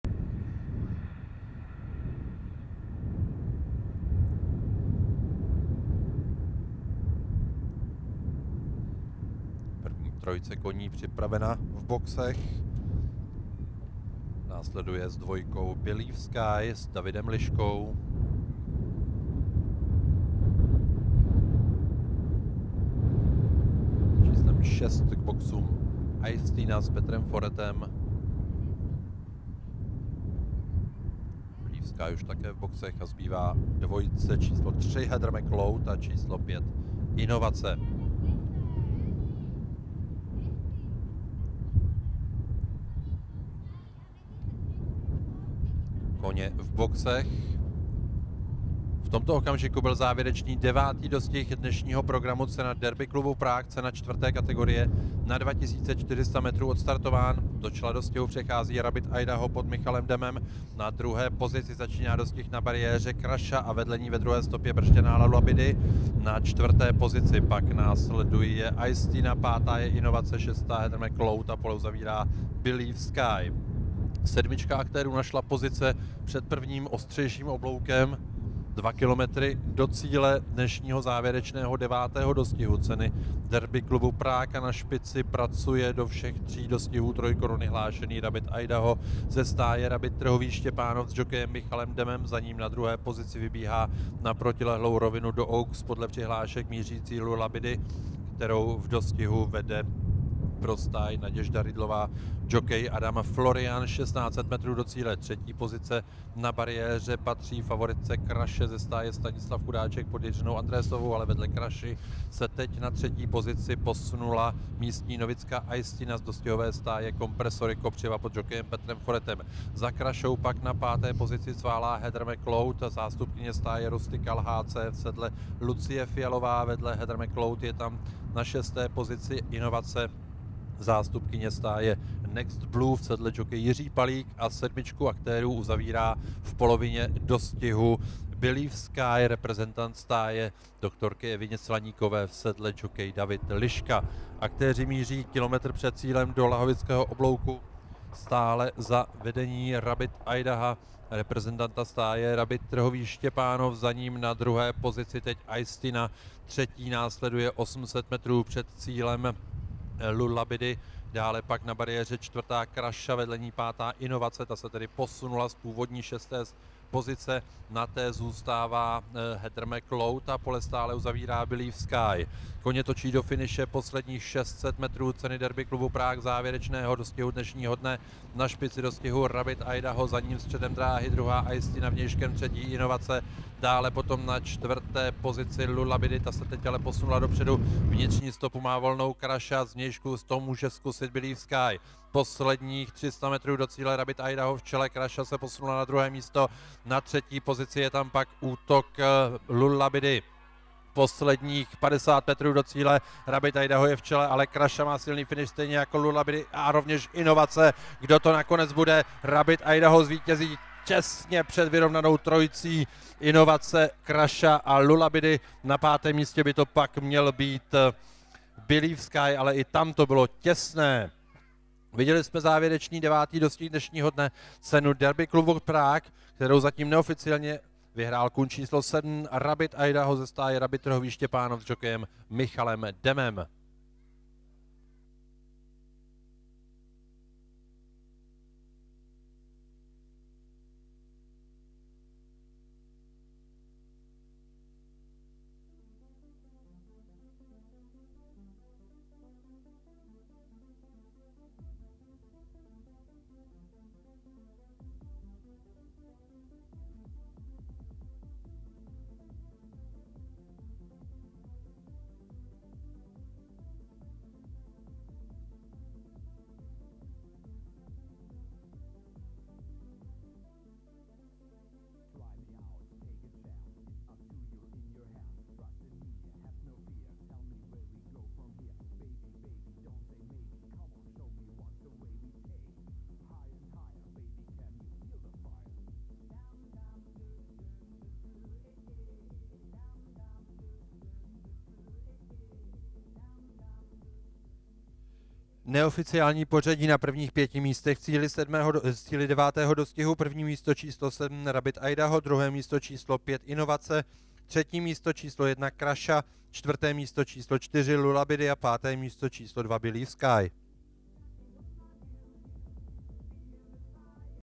Dostihy Chuchle Arena Praha 21. 4. 2024 - 9. dostih - Cena Derby Clubu Prague
Druhým mítinkem pokračovala v neděli 21. dubna letošní sezona v Chuchle Areně Praha. V Memoriálu profesora Michala se sešli tříletí, Memoriál Jaroslava Maška přinesl další souboj elitních sprinterů. EquiTV vám nabízí záznamy všech devíti dostihů.